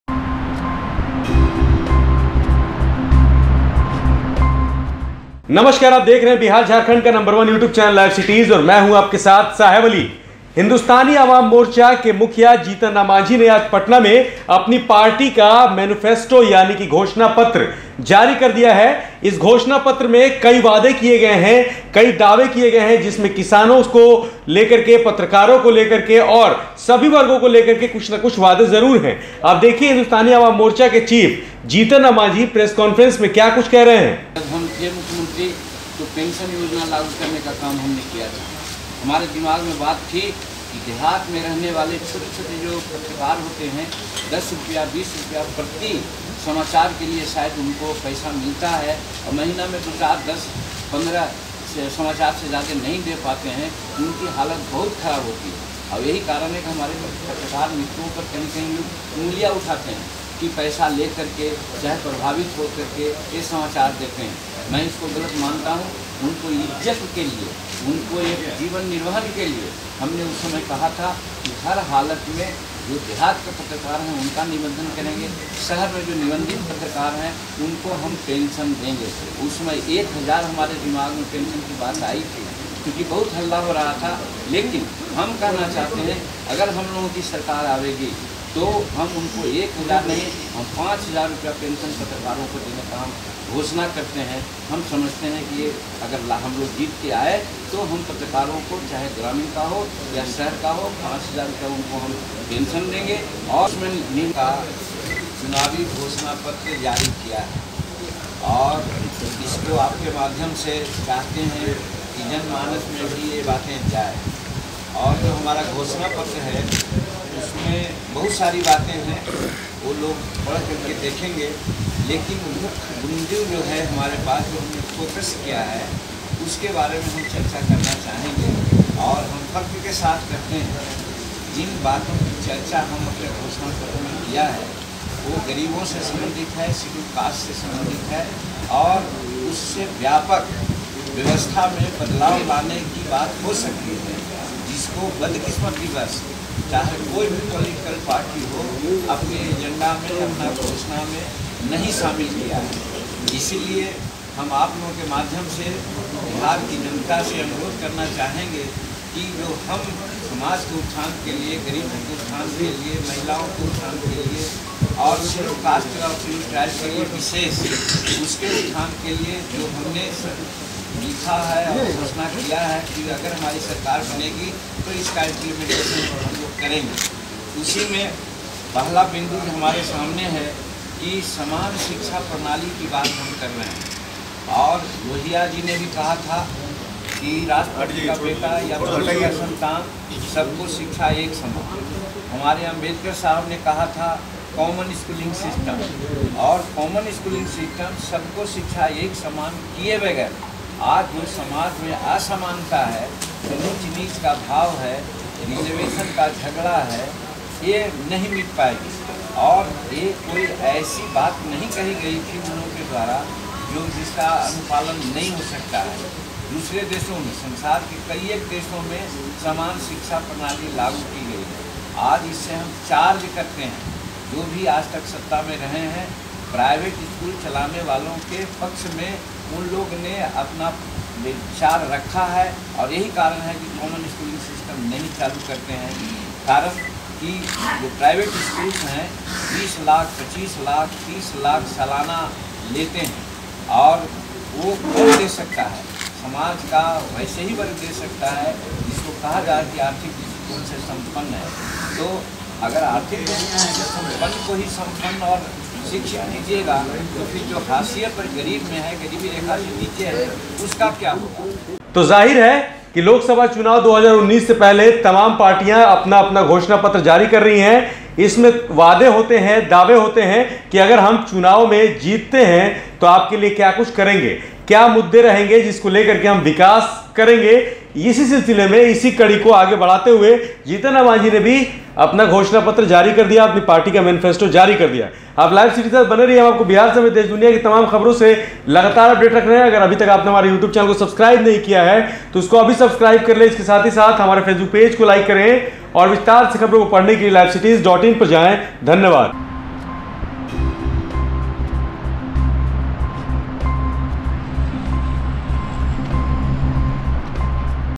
Lok Sabha Election को लेकर Jitan Ram Manjhi ने जारी किया Manifesto, जानिए क्या-क्या किए गए हैं वादे
Lok Sabha Election को लेकर Jitan Ram Manjhi ने जारी किया Manifesto, जानिए क्या-क्या किए गए हैं वादे | LiveCities